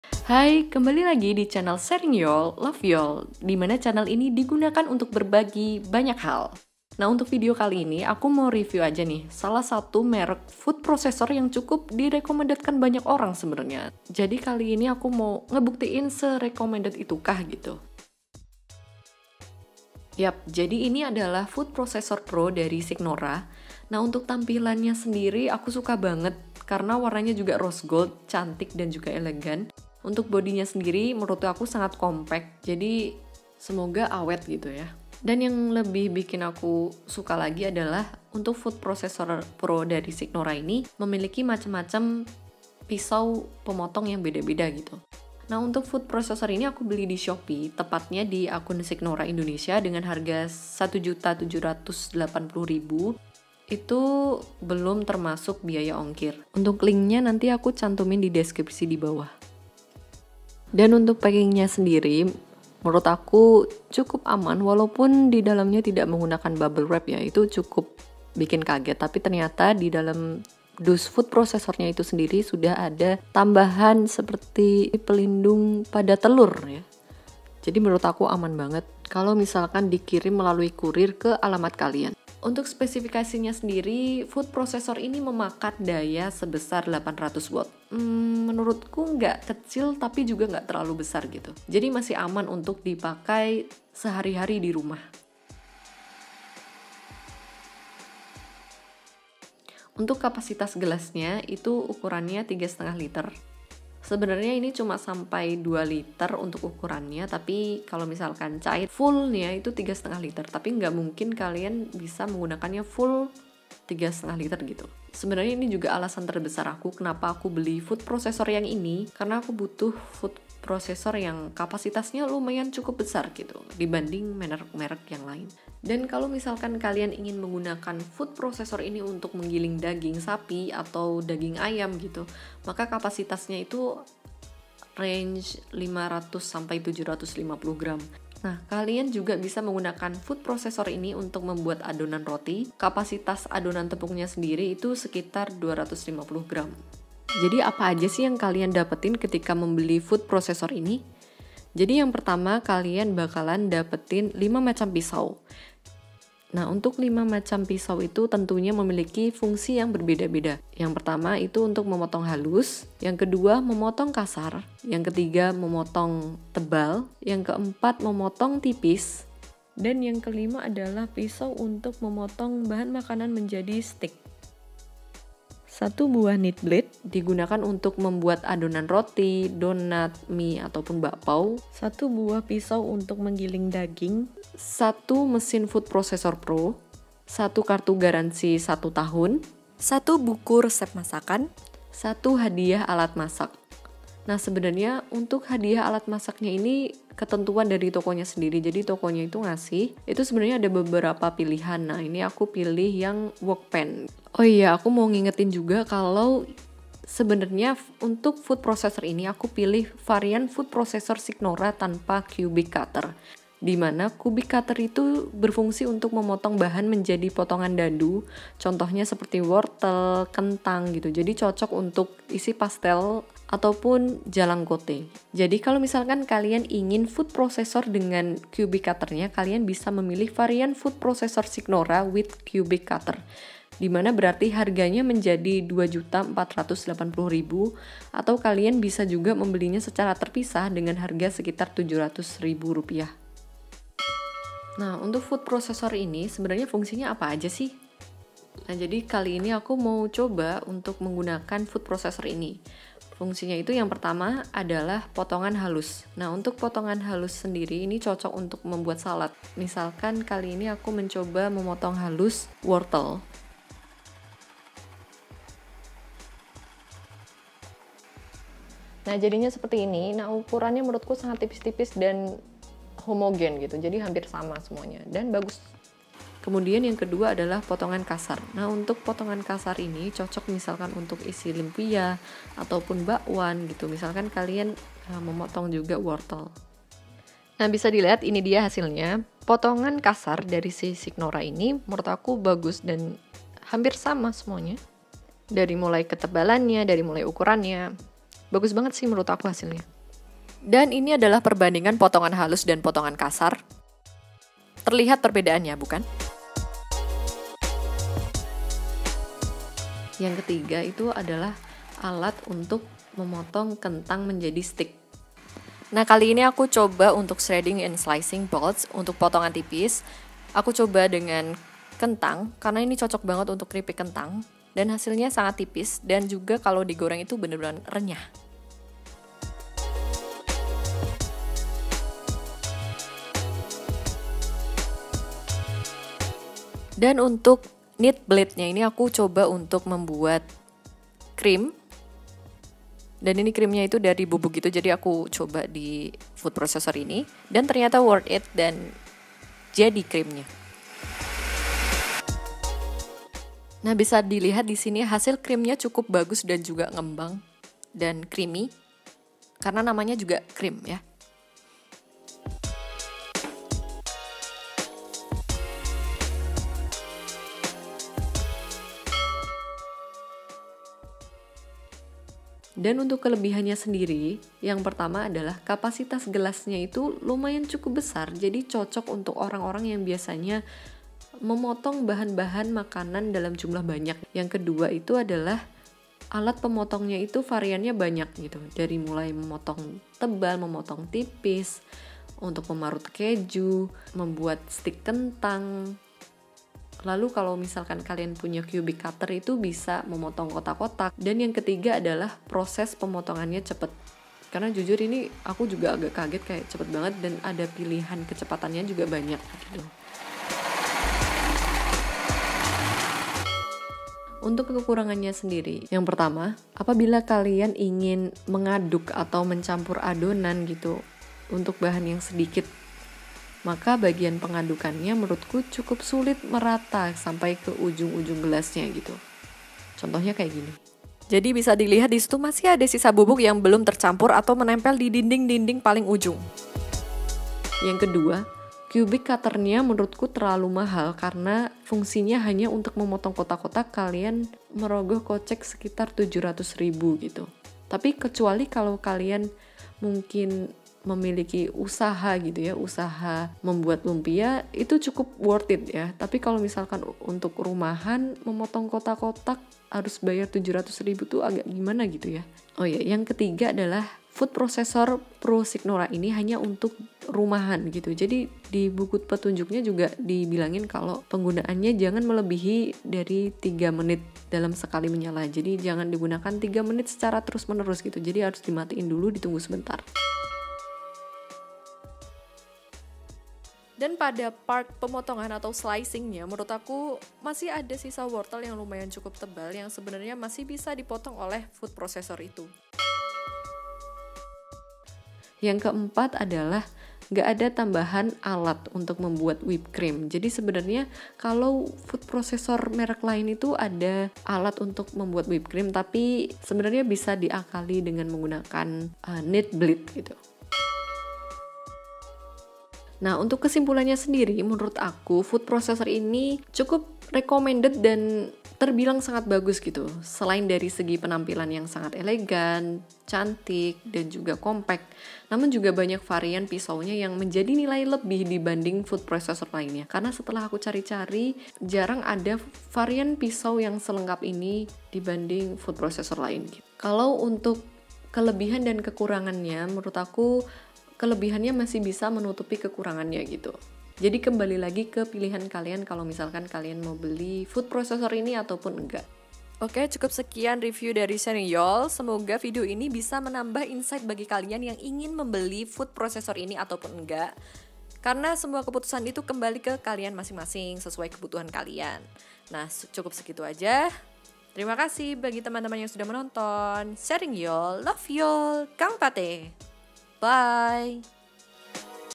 FOOD PROCCESOR PRO SIGNORA RECOMENDED Recomended |Bahas Tuntas 100%
REVIEW JUJUR FOOD PROCCESOR PRO SIGNORA MULTIFUNGSI| Bahas Tuntas 100%
FOOD PROCESSOR PRO SIGNORA

Tanpa Cubic Cutter
power : 800 watt, kapasitas daging : 500-700 gr, kapasitas gelas : 35 ltr
, adonan donat/mie : 250gr

Terdapat 5macam pisau :
- memotong halus
- potongan kasar
- potongan tebal
- potongan tipis
- potongan  agak tebal

blade ada 2 :
- stainless steel untuk aneka bakso
- kneed blade untuk adonan roti/donat/mie/bakpao

yang akan diterima 1 box
- 1x Mesin food processor Pro
- 1x kartu garansi 1 tahun
- 1x hadiah kategori 6
- 1x aneka resep signora